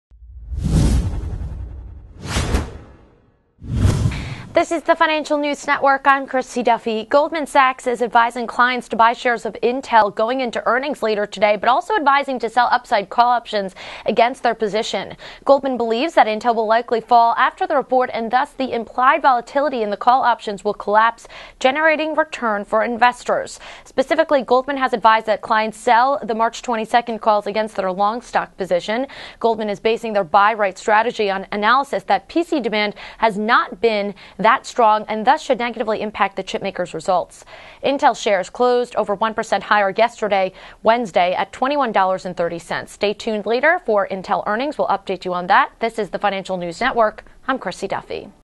Goldman Advises to Sell Call Options on Intel (INTC)
Goldman Sachs is advising clients to buy shares of Intel (NASDAQ:INTC) going into earnings later today but also advising to sell upside call options against their position. Goldman believes that Intel will likely fall after the report and thus the implied volatility in the call options will collapse generating return for investors. Specifically, Goldman has advised that clients sell the March 22 calls against their long stock position.

Goldman is basing their buy-write strategy on analysis that PC demand has not been that strong and thus should negatively impact the chipmaker's results.

Intel shares closed 1.2% higher yesterday at $21.30.